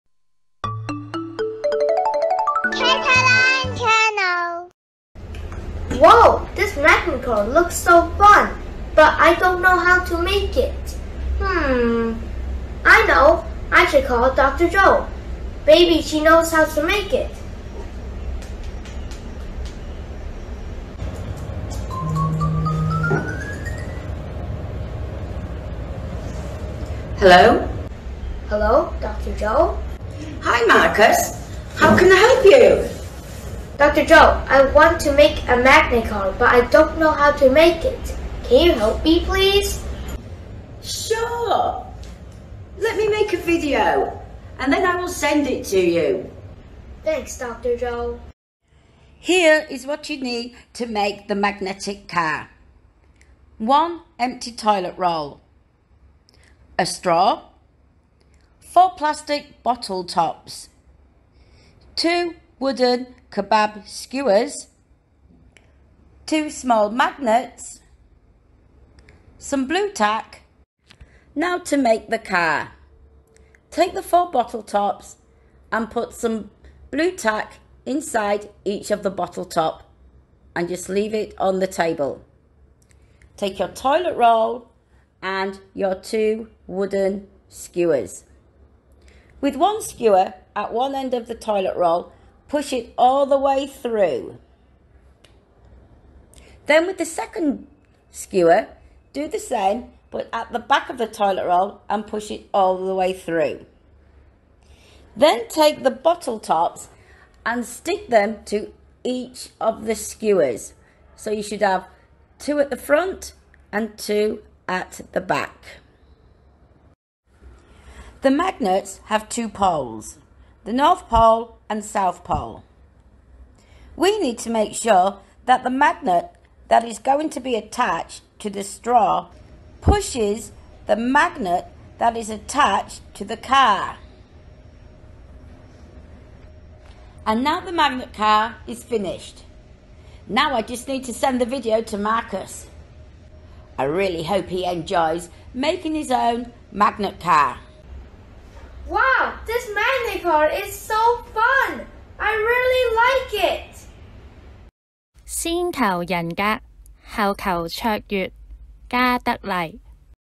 Magnet Car
咦？Marcus跟Dr. Jo學整的磁石車🚗好像很有趣，小朋友，你們也有興趣學嗎？不如我們一起去片，學習如何用簡單的材料去弄一架磁石車😉

The magnet car 🚗 Marcus made looks very interesting. Children, do you want to make your own magnet car? Let's learn it from Dr. Jo!